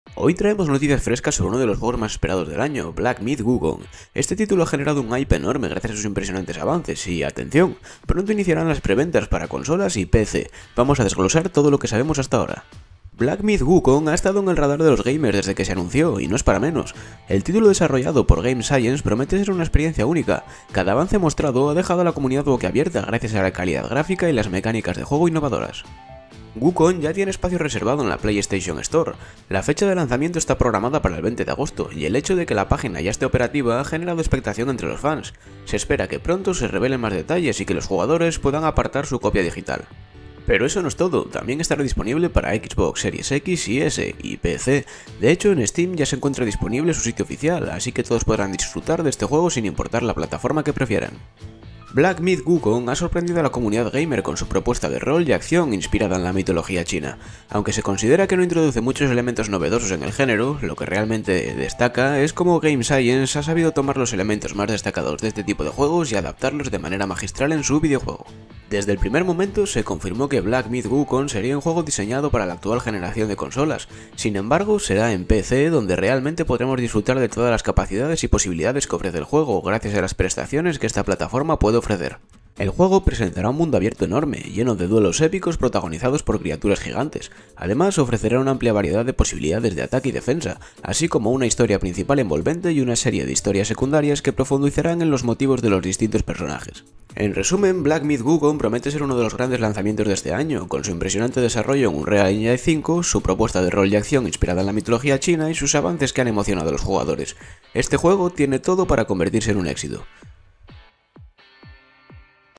💛 Black Myth Wukong en Preventa 💛
Hoy les traemos noticias frescas sobre uno de los juegos más esperados del año: ¡Black Myth: Wukong! Este título ha generado un hype enorme gracias a sus impresionantes avances y, atención, ¡pronto iniciarían las preventas para consolas y PC! Vamos a desglosar todo lo que sabemos hasta ahora.
Black Myth: Wukong y su desarrollo con Unreal Engine 5
Black Myth: Wukong ha estado en el radar de los gamers desde que se anunció, y no es para menos. El título desarrollado por Game Science promete ser una experiencia única. Cada avance mostrado ha dejado a la comunidad boquiabierta, gracias a la calidad gráfica y las mecánicas de juego innovadoras.
Fecha de lanzamiento y detalles de preventa
Wukong ya tiene su espacio reservado en la PlayStation Store. La fecha de lanzamiento está programada para el 20 de agosto, y el hecho de que la página ya esté operativa ha generado expectación entre los fans. Se espera que pronto se revelen más detalles y que los jugadores puedan apartar su copia digital.
Pero eso no es todo, ¡también estará disponible para Xbox Series X|S y PC! De hecho, en Steam ya se encuentra disponible su sitio oficial, así que todos podrán disfrutar de este juego sin importar la plataforma que prefieran.
La propuesta de Black Myth: Wukong
Black Myth: Wukong ha sorprendido a la comunidad gamer con su propuesta de rol y acción inspirada en la mitología china. Aunque se considera que no introduce muchos elementos novedosos en el género, lo que realmente destaca es cómo Game Science ha sabido tomar los elementos más destacados de este tipo de juegos y adaptarlos de manera magistral en su videojuego.
Los avances que emocionan a los jugadores
Desde el primer momento, se confirmó que Black Myth: Wukong sería un juego diseñado para la actual generación de consolas. Sin embargo, será en PC donde realmente podremos disfrutar de todas las capacidades y posibilidades que ofrece el juego, gracias a las prestaciones que esta plataforma puede ofrecer.
El juego presentará un mundo abierto enorme, lleno de duelos épicos protagonizados por criaturas gigantes. Además, ofrecerá una amplia variedad de posibilidades de ataque y defensa, así como una historia principal envolvente y una serie de historias secundarias que profundizarán en los motivos de los distintos personajes.
Conclusión y expectativas
En resumen, Black Myth: Wukong promete ser uno de los grandes lanzamientos de este año. Con su impresionante desarrollo en Unreal Engine 5, su propuesta de rol y acción inspirada en la mitología china y sus avances que han emocionado a los jugadores, este juego tiene todo para convertirse en un éxito.
Si eres fan de los juegos de rol y acción, definitivamente querrás apartar tu copia de Black Myth: Wukong y estar listo para sumergirte en este mundo épico lleno de aventuras y combates espectaculares.